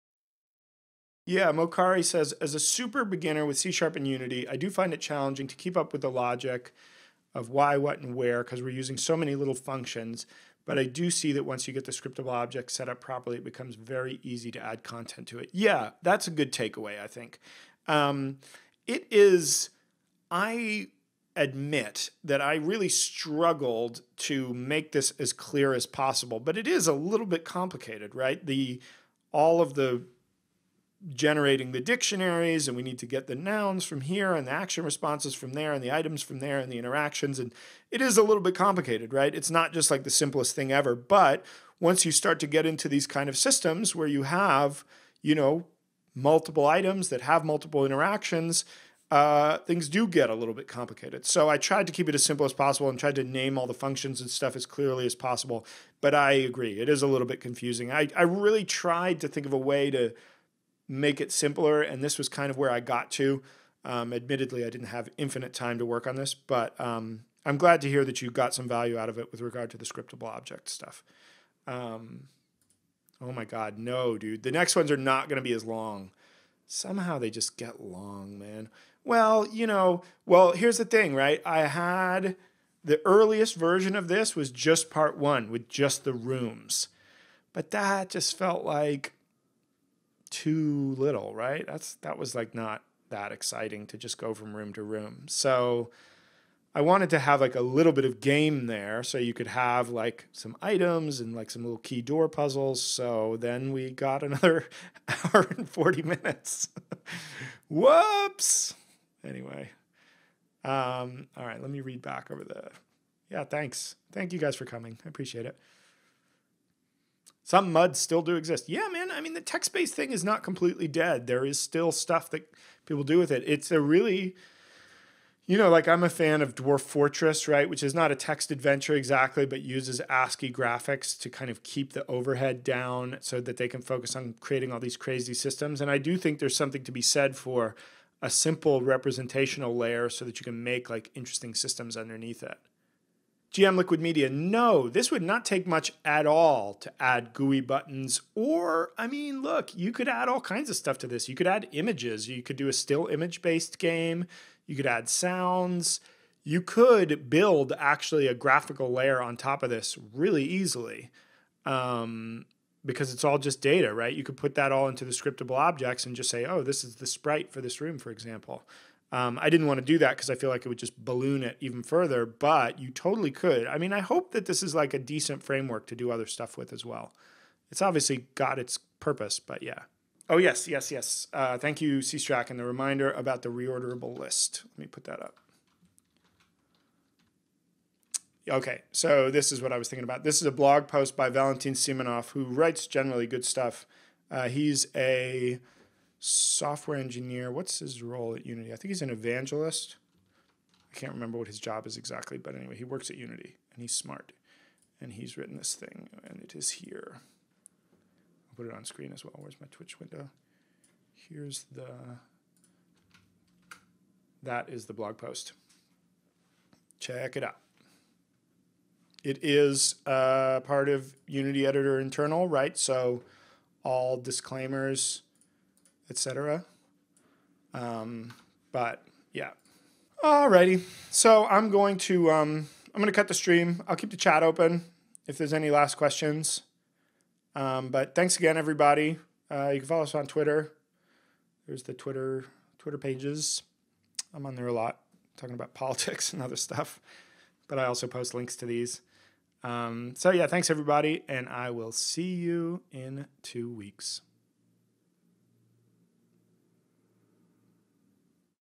Creating a Text Based Adventure Part 2 - Displaying Inventory [10/10] Live 2017/3/29
In this second session we will continue to learn how to program a text based adventure game in C# by adding items which can be examined, taken and used, along with a very simple inventory system. In this episode we will finish the session by taking questions and answers.